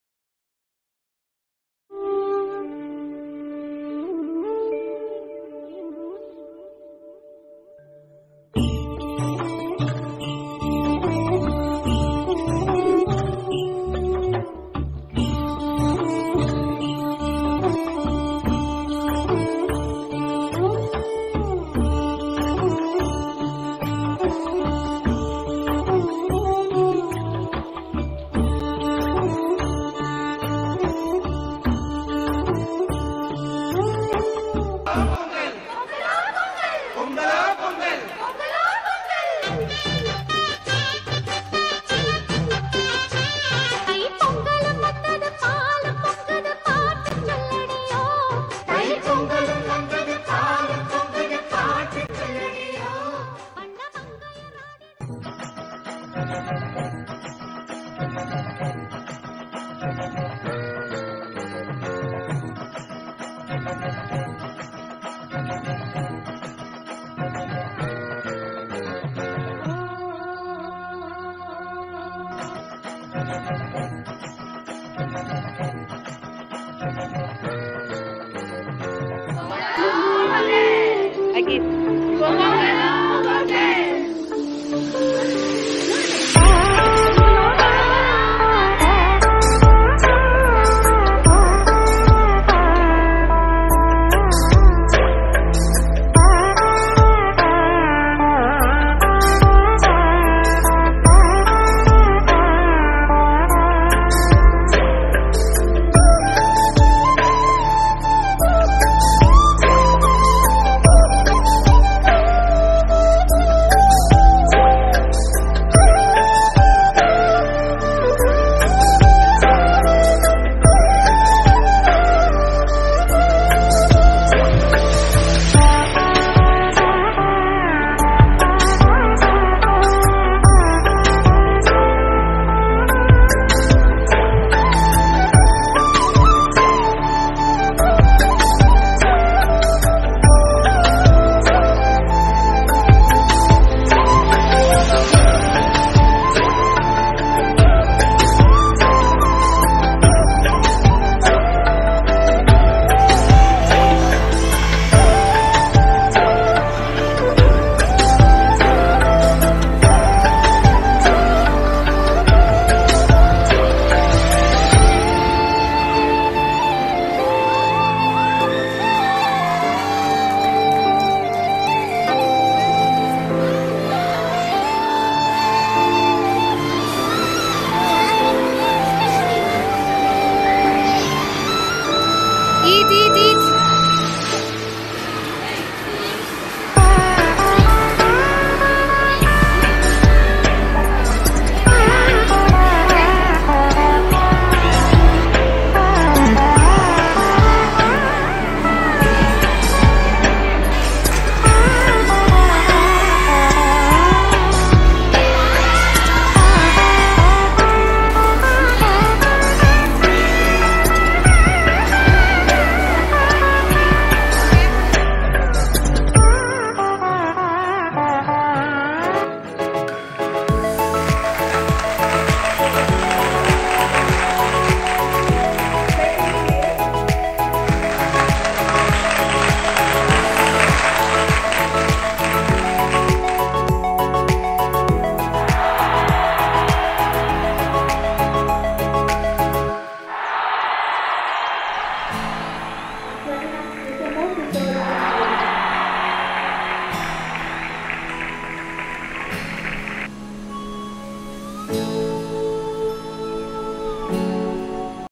Our Lady's Centre Matriculation School-Broadway | Pongal Festival Celebration.
Celebration of pongal festival and Awarding of Guides Boscoree students.
May the pongal festival which makes the begining of the harvest season brings you joy prosperity and wealth to you and your family.